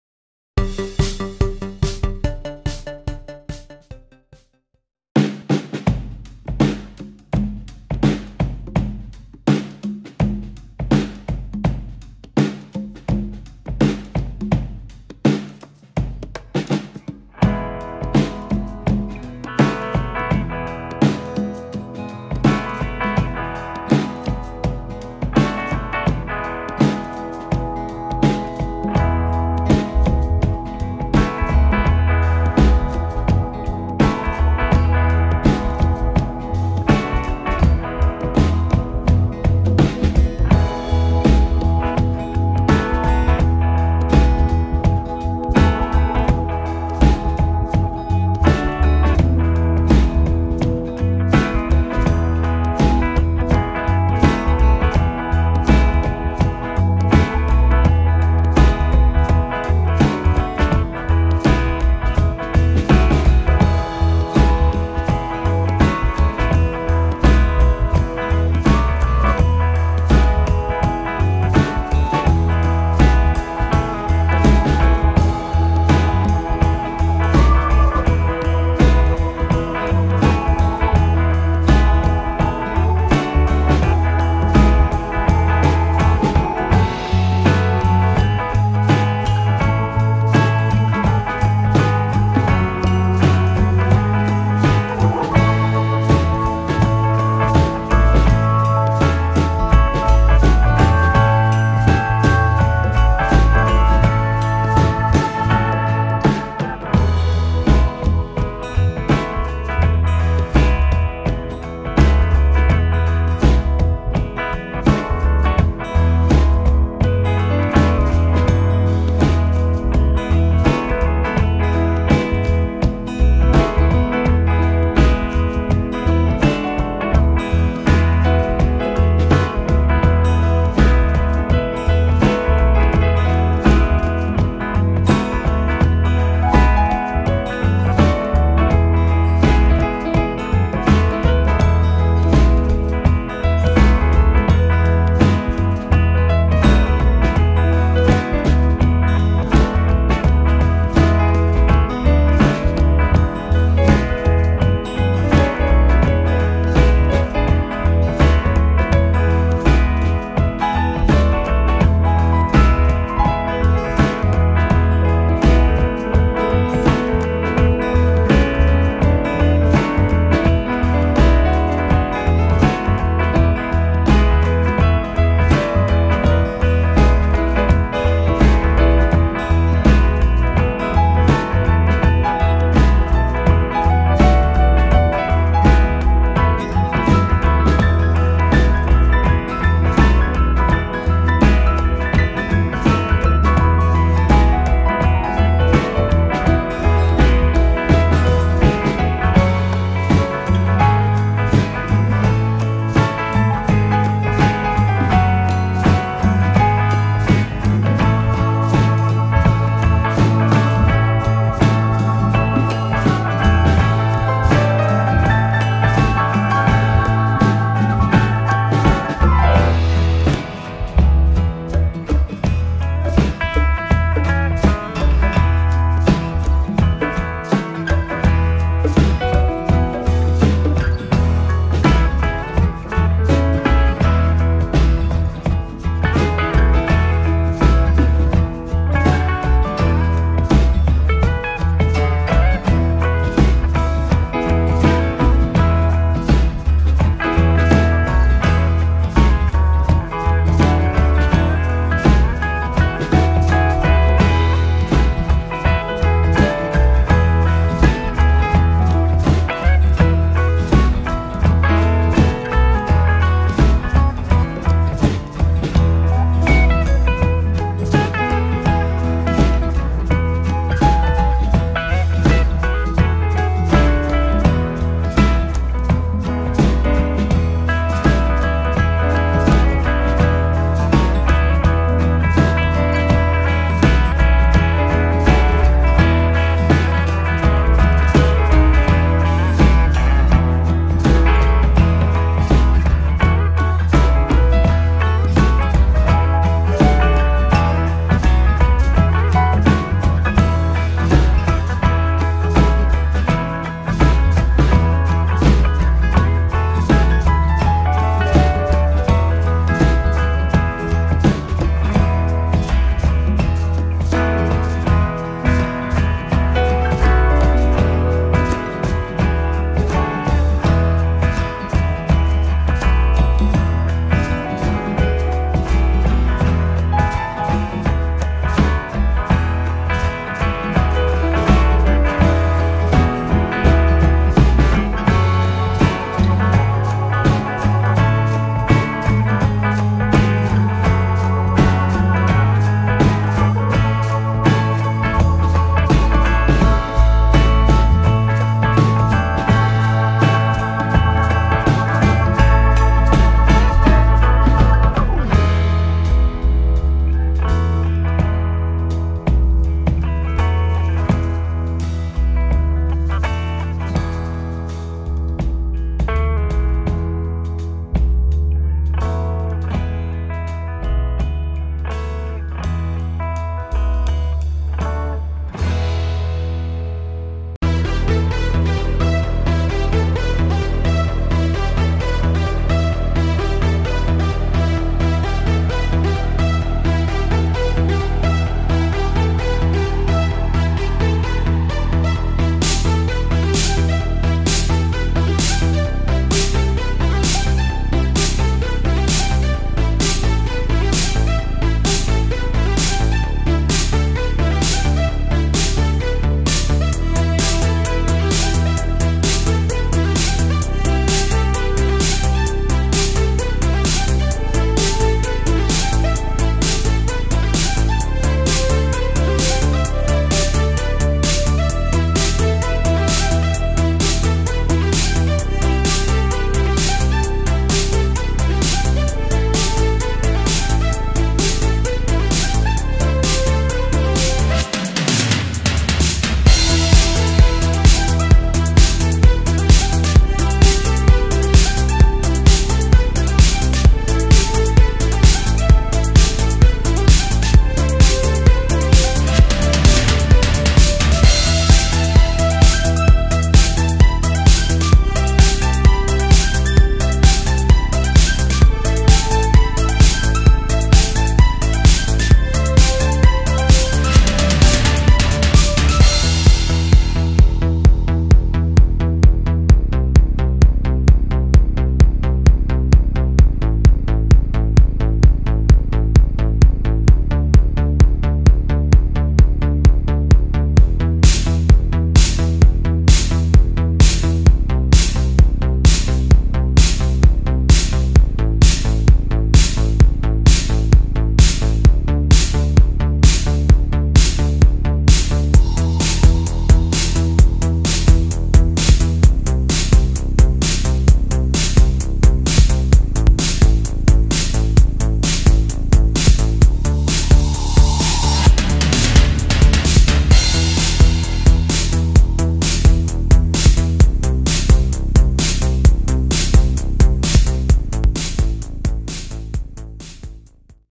Vase Painting with enamel synthetic Paints and purgaza brushes
This video is about base painting.
First of all i select a pot from market and I  prepare the surface for Paint white .
Marking and fill the paint in every boxes i show in this video.